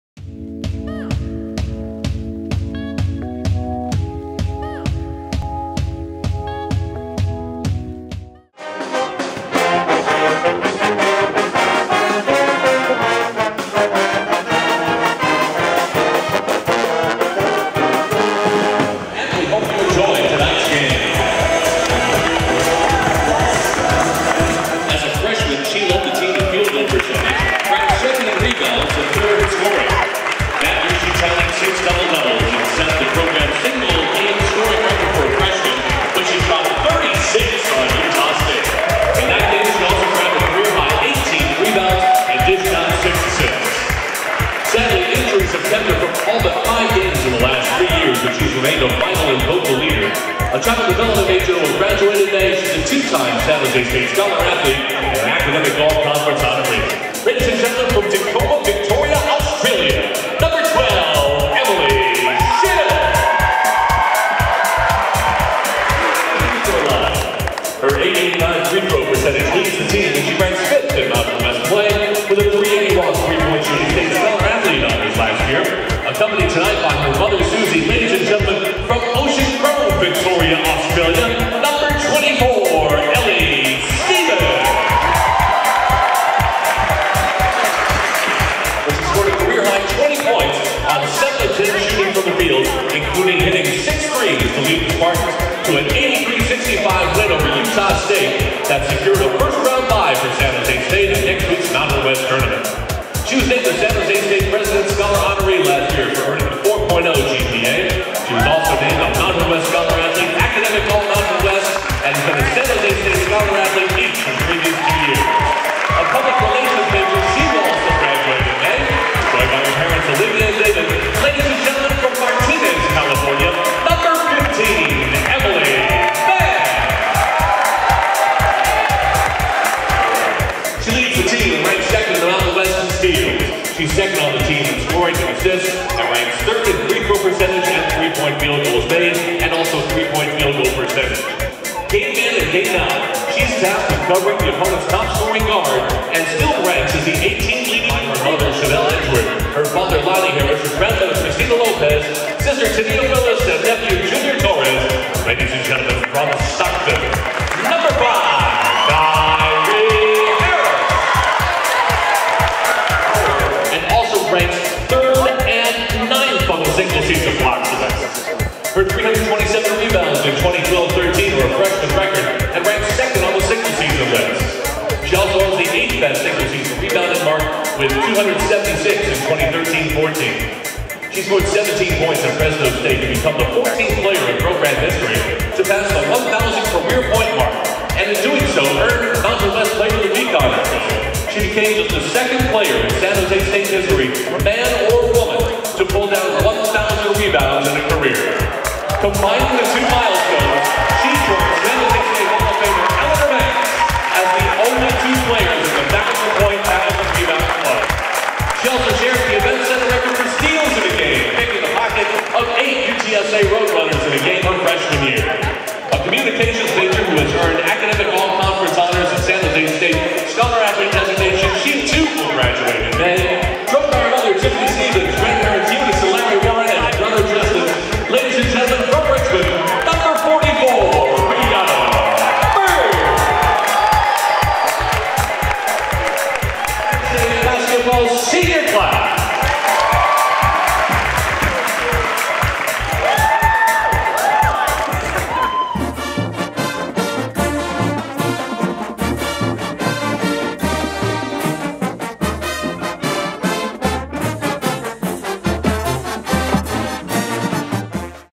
San Jose State Women's Basketball Senior Night (3-1-16)
San Jose, Calif. - In its final home game of the 2015-16 Mountain West schedule, the San José State Spartans host the Wyoming Cowgirls. The game will mark the final appearance for five seniors, Emily Schill, Ellie Stevens, Emily Vann, Nyre Harris and Riana Byrd.